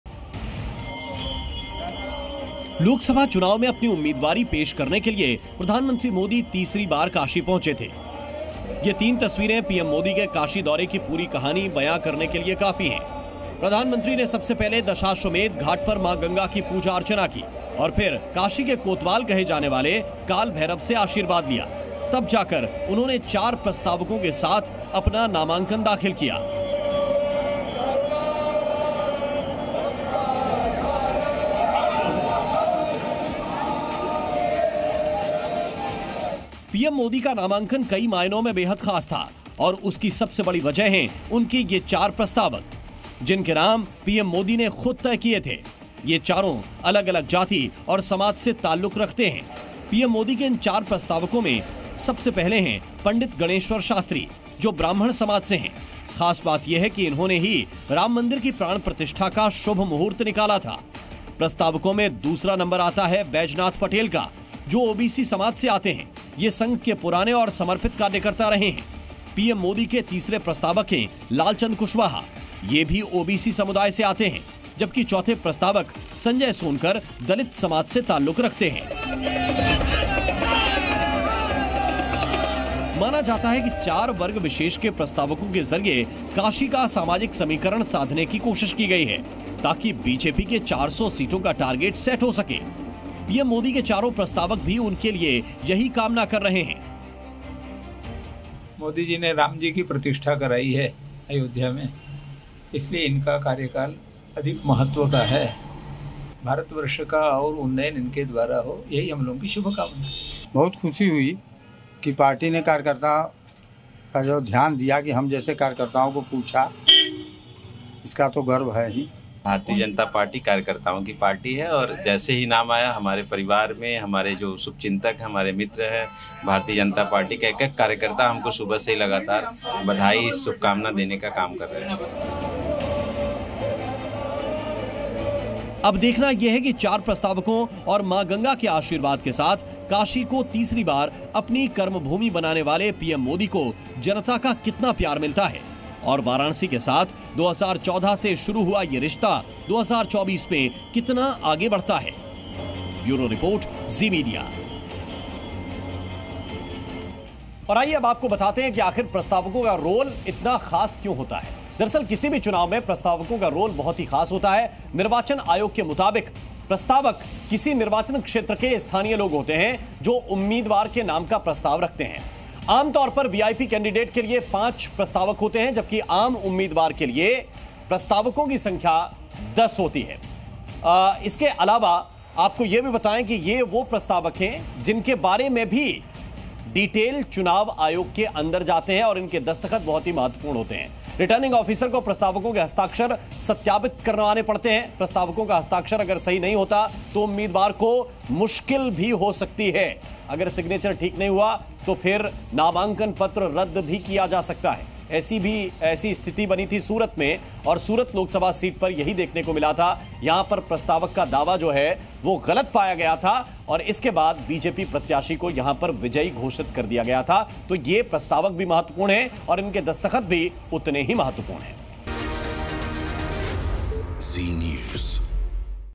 Rajneeti: पीएम मोदी के 4 सारथी | PM Modi Nomination | Lok Sabha Election 2024 | Hindi News Update
Rajniti: प्रधानमंत्री नरेंद्र मोदी ने वाराणसी से तीसरी बार नामांकन कर दिया है. मंगलवार 14 मई को पीएम मोदी ने काल भैरव के दर्शन करने के बाद नामांकन पत्र दाखिल किया. इस दौरान गृहमंत्री अमित शाह, रक्षा मंत्री राजनाथ सिंह सहित कई पार्टियों के चीफ और कई राज्यों के मुख्यमंत्री भी मौजूद रहे.

Rajniti: Prime Minister Narendra Modi has filed nomination for the third time from Varanasi. On Tuesday, May 14, PM Modi filed nomination papers after having darshan of Kaal Bhairav. During this, Home Minister Amit Shah, Defense Minister Rajnath Singh and chiefs of many parties and Chief Ministers of many states were also present.